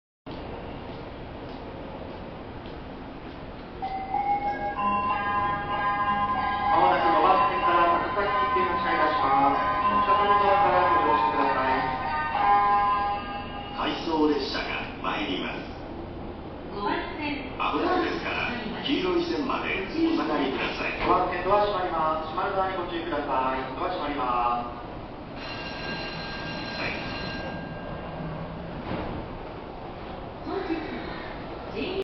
JR東日本 上野駅 5番線発車メロディー
JR東日本上野駅5番線の発車メロディーです。